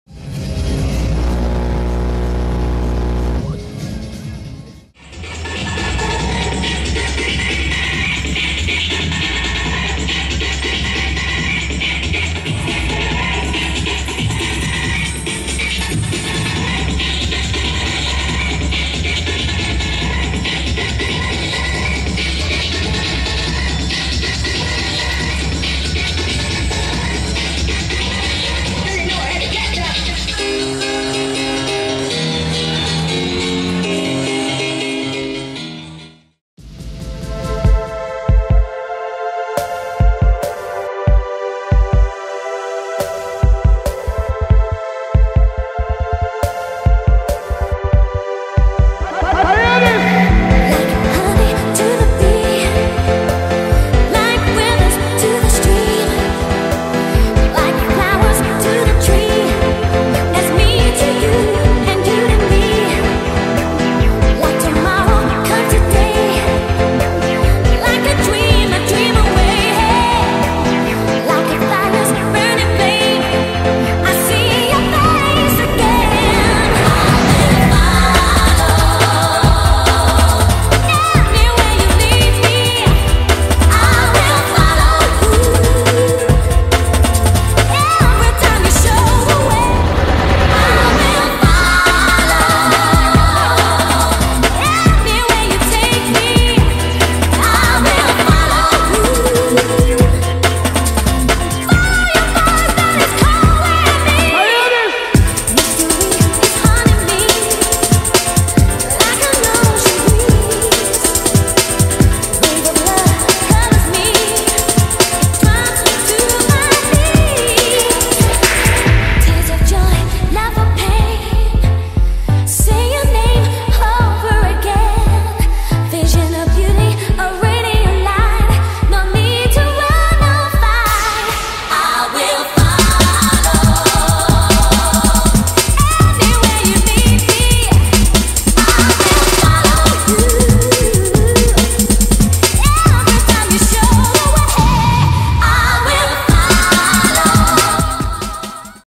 III Tuning San Pedro Puertito de Güimar
Evento desarrollado en Plaza Las Indias el pasado Sábado.
también se desarrollo el Güímar Dj's 3.0